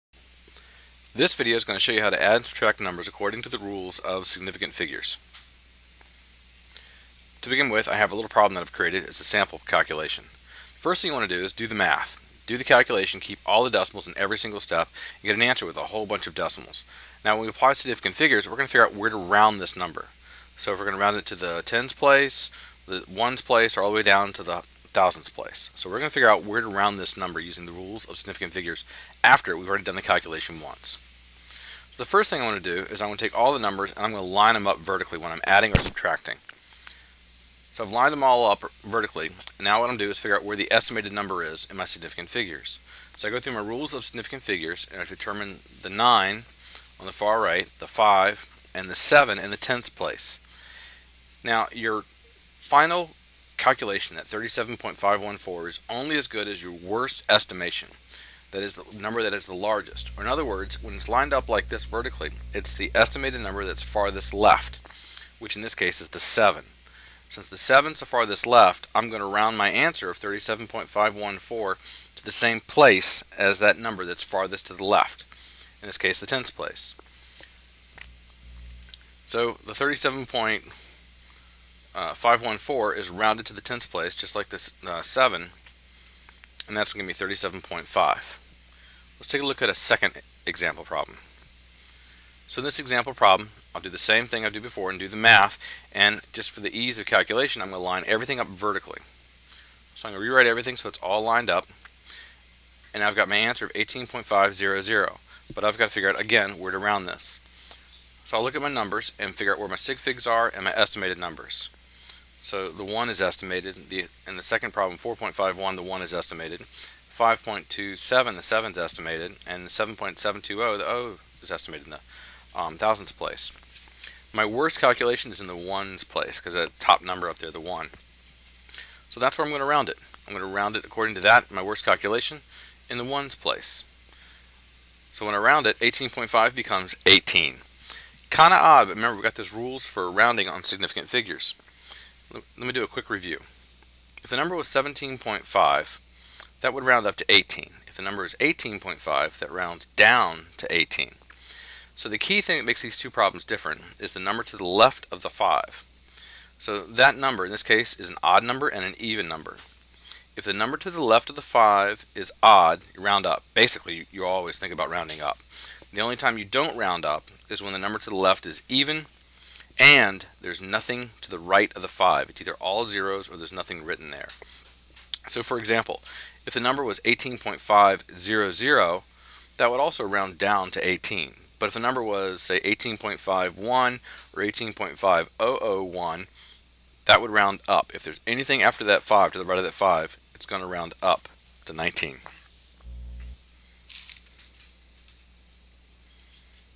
Significant Figures Addition and Subtraction Example
This video describes how to apply the rules of significant figures to addition, subtraction, and rounding through some example problems.